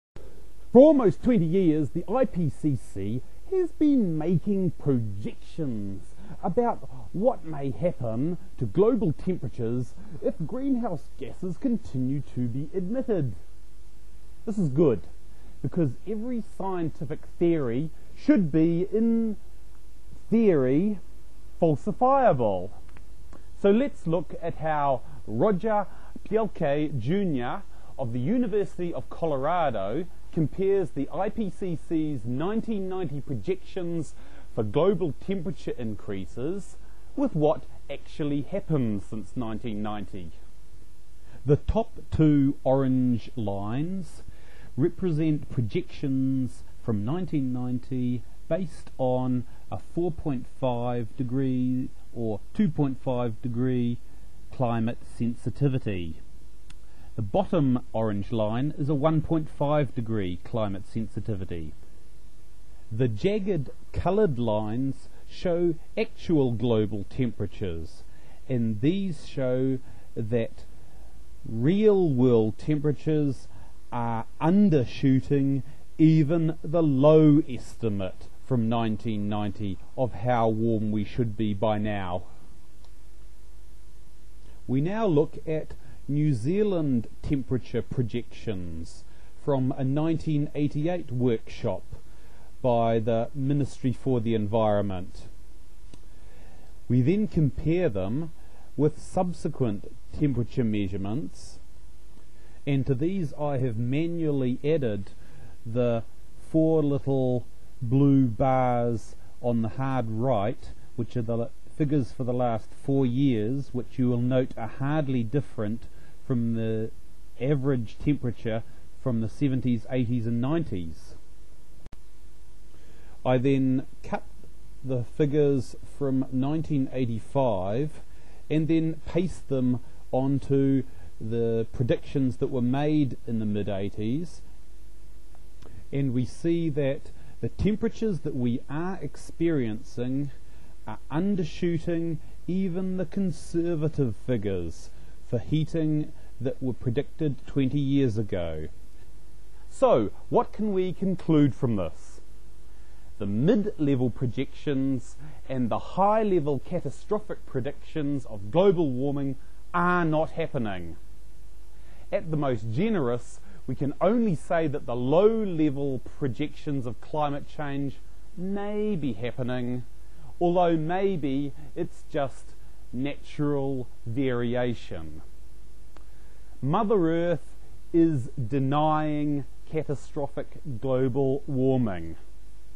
Mother Earth: Global Warming Denier
For almost 20 years the IPCC has been making "projections" about what may happen to global temperatures if GHG continue to be emitted. This is good, because every scientific theory should be theoretically falsifiable. So lets look at how Roger Pielke Jr of the University of Colorado compares the  IPCC's 1990 projections for temperature increases.